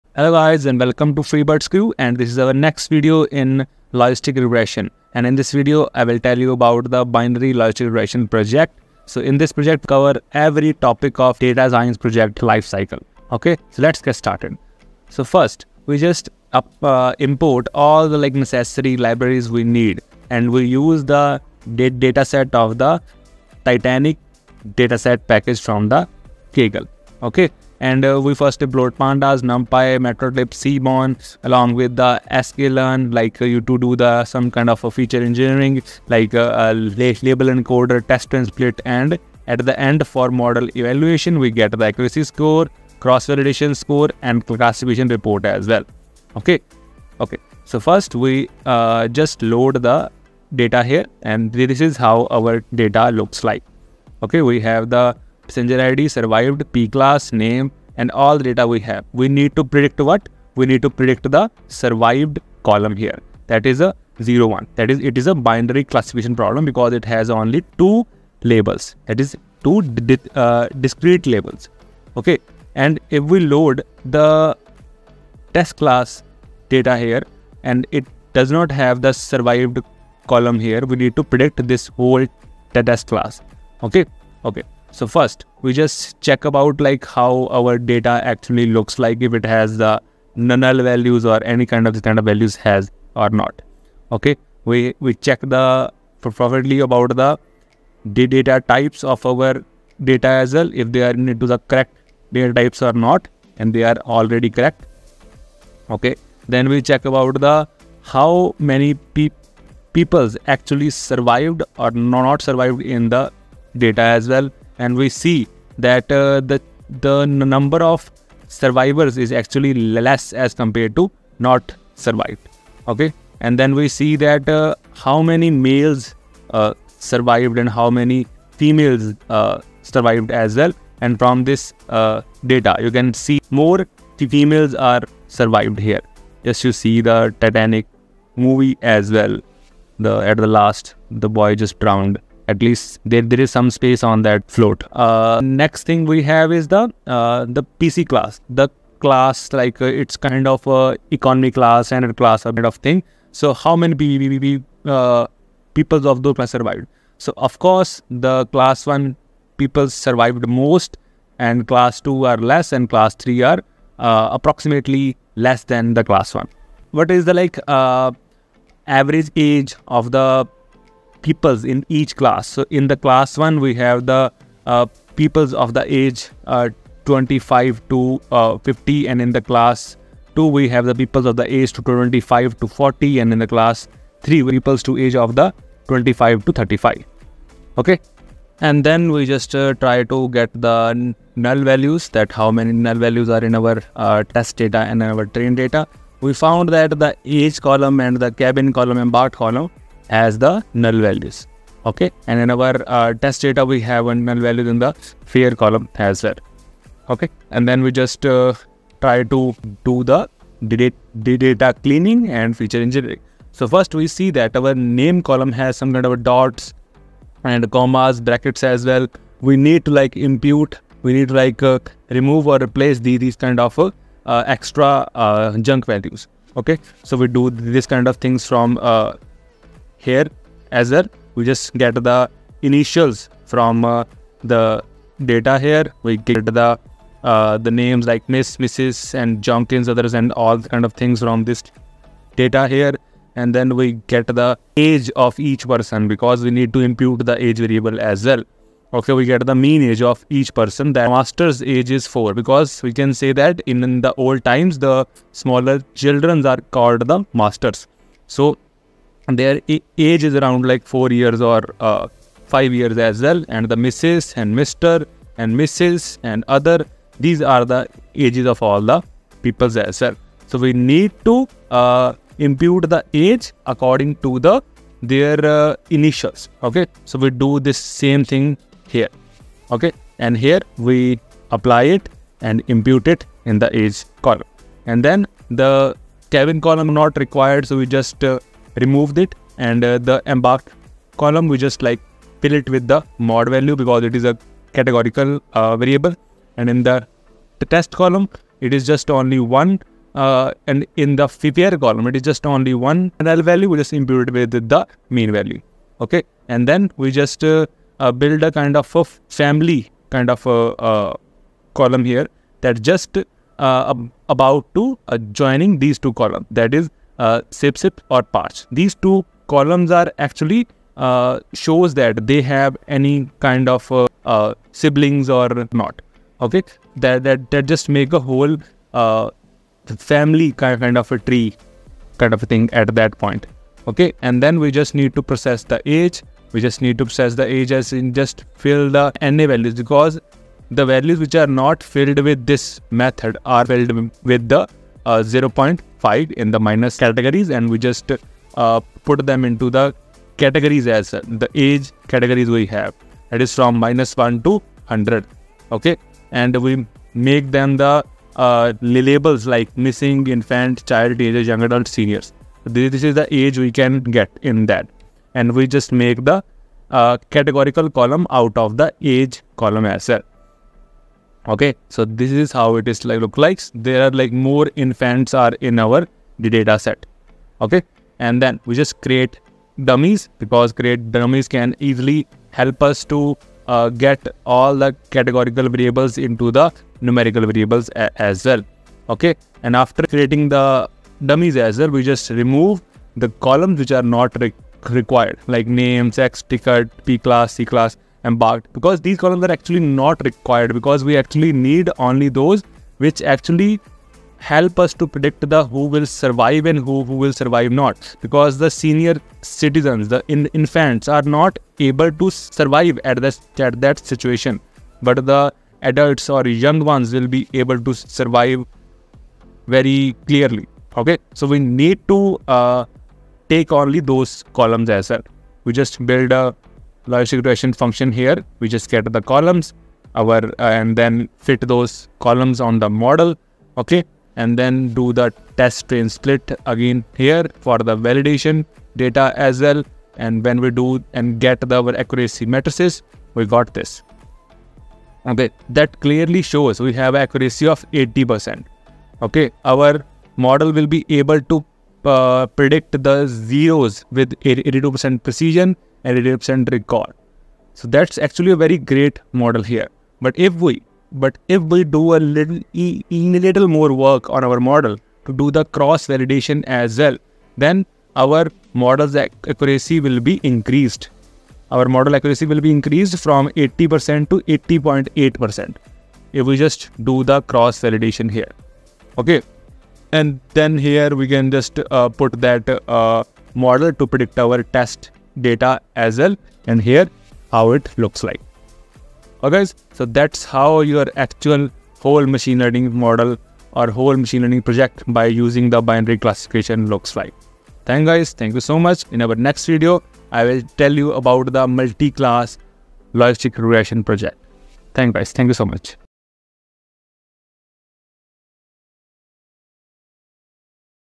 Logistic Regression Project [ Explained ] Binary Classification | Data Science Project | Python
In this video, I discussed Logistic Regression, I explained the Full Logistic Regression Project for Binary Classification Problems with a Complete Data Science Project Life Cycle. This Video shows, How I load the data and do Data Cleaning, Feature Engineering, Feature Selection, Machine Learning Model Building, and Model Validation with Testing Data Validation. So This Project really helps you to crack data science interviews.